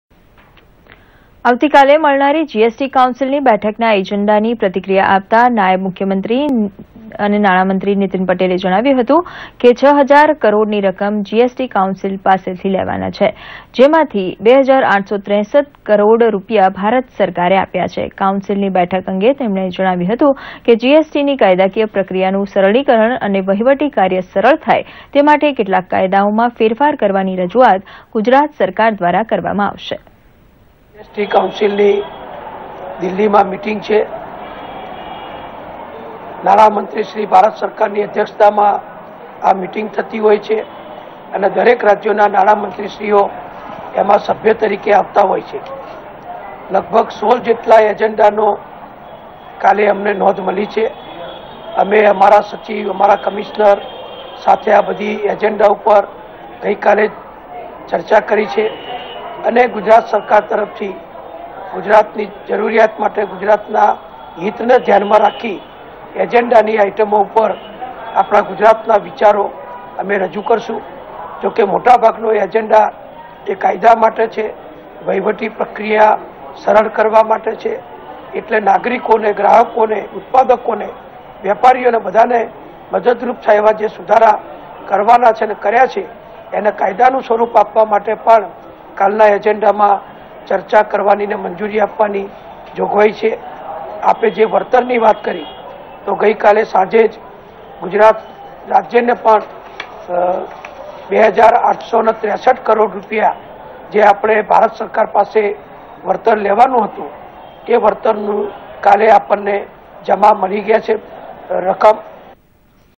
આવતીકાલે GST કાઉન્સીલની યોજાશે બેઠક
આવતીકાલે મળનારી GST કાઉન્સીલની બેઠકના એજન્ડાની પ્રતિક્રિયા આપતા નાયબ મુખ્યમંત્રી, નાણામંત્રી નીતિન પટેલે જણાવ્યું હતું કે ,6000 કરોડની રકમ GST કાઉન્સીલ પાસેથી લેવાના છે. જેમાથી 2863 કરોડ રૂપીયા ભારત સરકારે આપ્યા છે. કાઉન્સીલની બેઠક અંગે તેમણે જણાવ્યું હતું કે GST ની કાયદાકિય પ્રક્રિયાનું સરણીકરણ અને વહીવટી કાર્યસરળ થાય તે માટે કેટલાક કાયદાઓમાં ફેરફાર કરવાની રજુઆત ગુજરાત સરકાર દ્વારા કરવામાં આવશે.